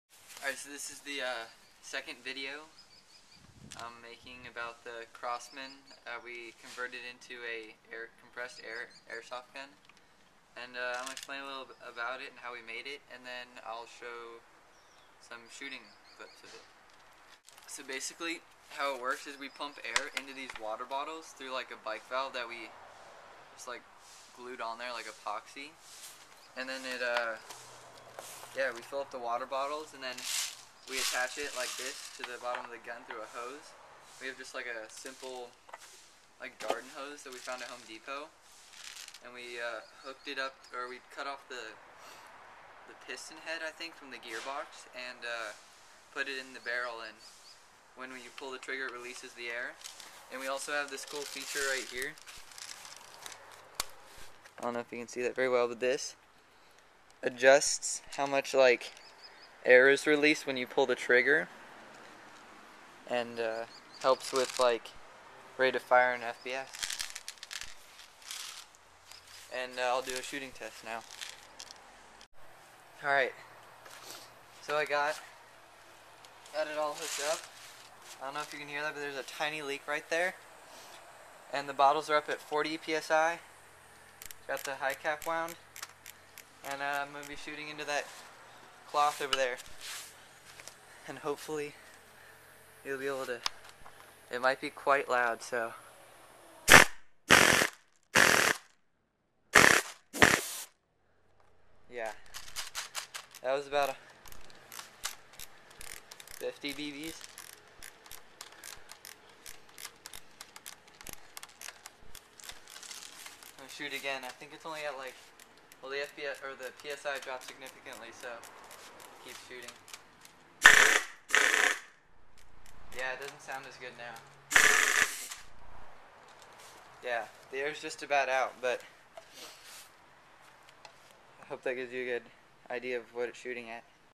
Crosman Converted to Compressed Air Airsoft Gun explanation
my friend and I converted my old pulse R72 into a compressed airsoft gun running on 2 soda bottles at 40 psi
-shooting test and explanation of how it works